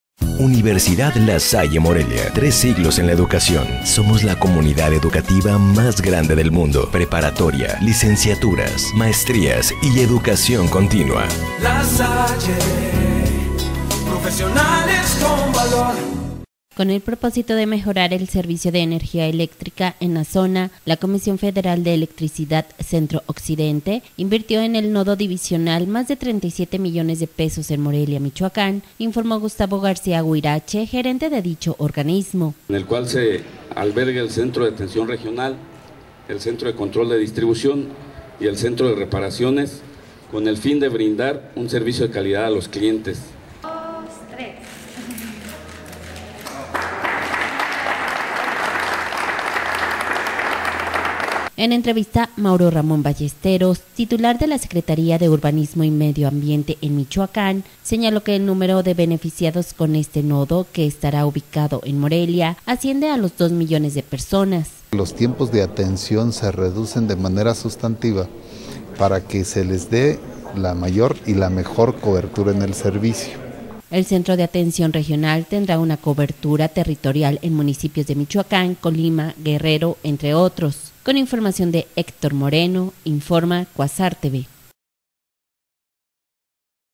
Inaugura CFE Centro de Atención Regional en Morelia
Con el propósito de mejorar el servicio de energía eléctrica en la zona, la Comisión Federal de Electricidad Centro Occidente invirtió en el Nodo Divisional más de 37 millones de pesos en Morelia, Michoacán, informó Gustavo García Huirache, gerente de dicho organismo…